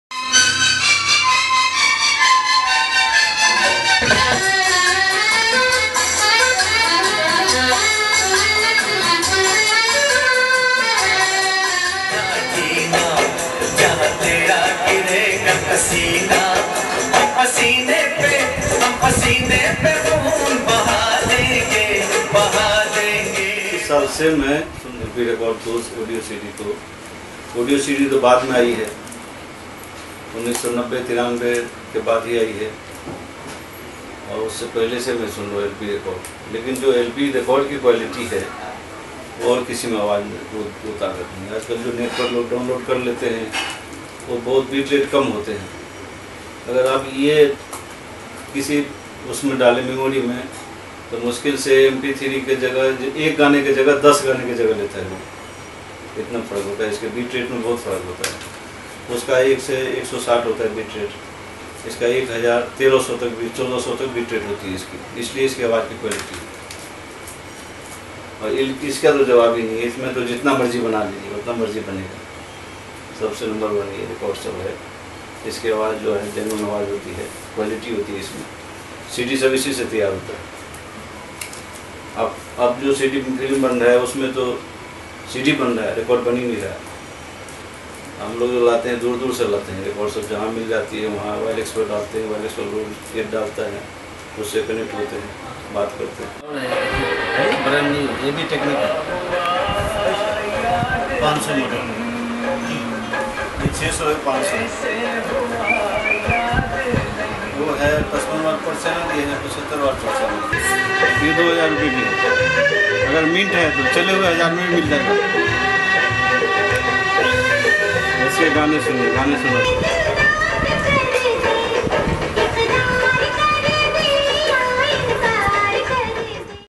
The Record Man
With the passage of time, records have become extinct and it's difficult to find them. It is a treasure trove for all the vintage music lovers. Harish Kumar is one among many music lovers who want their food for the soul on record.